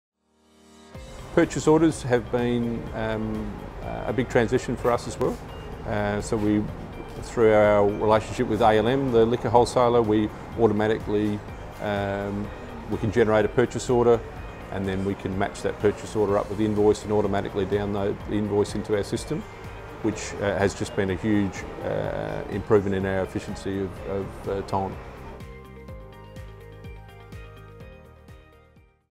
Purchase Orders Made Easy | The Arkaba Hotel - H&L Australia
Sam McInnes is the General Manager of the Hurley Hotel Group whose many high-profile sites include the iconic Arkaba Hotel in Fullarton.  Reduce data entry errors and improve efficiency with H&L’s solutions with purchase orders automatically generated and matched to the invoice and the invoice then automatically downloaded to the system.  Too easy!  Watch what Sam has to say about H&L’s clever solutions.